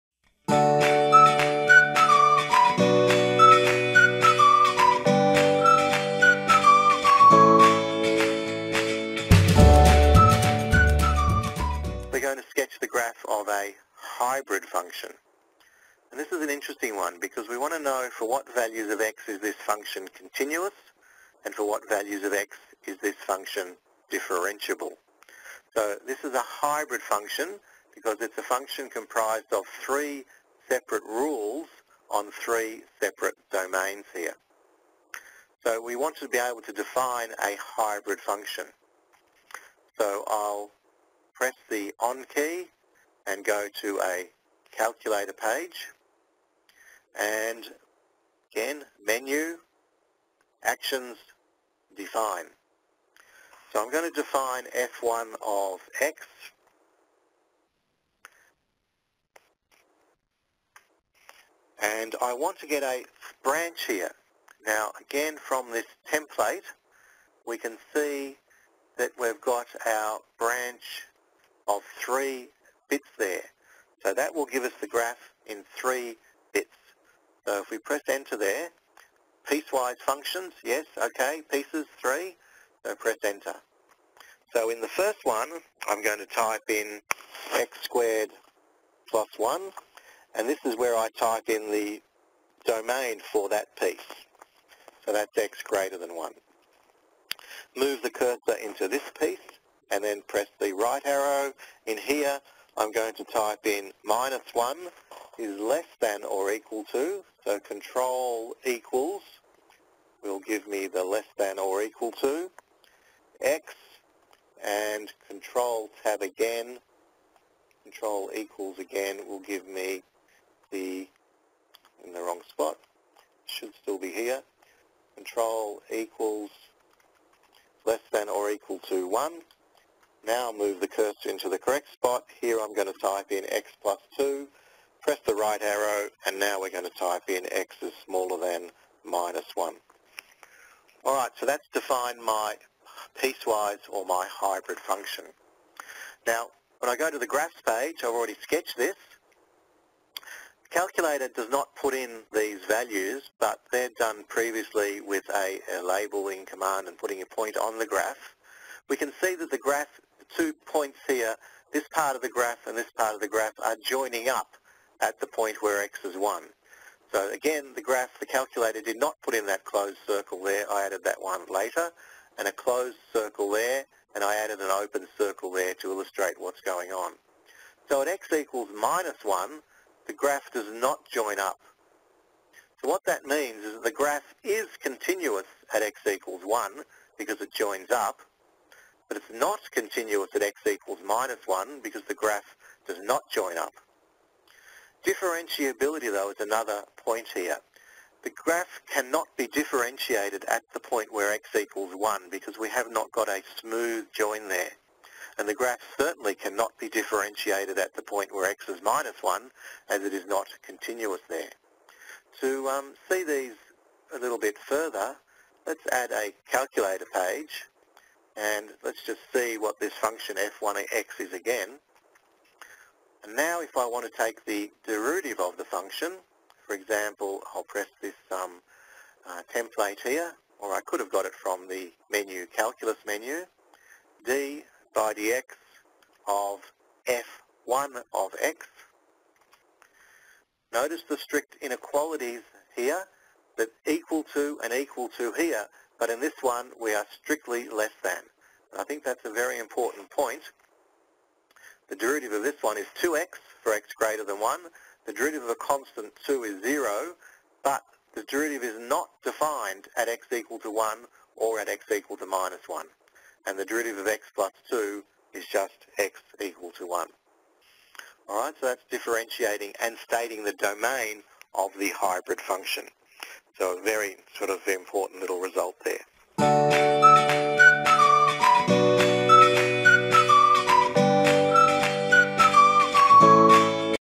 Hybrid Functions -Mathematical Methods - Exam Readiness with TI-Nspire
How to define a hybrid function, specify the domain and check for differentiability using the TI-Nspire CX CAS.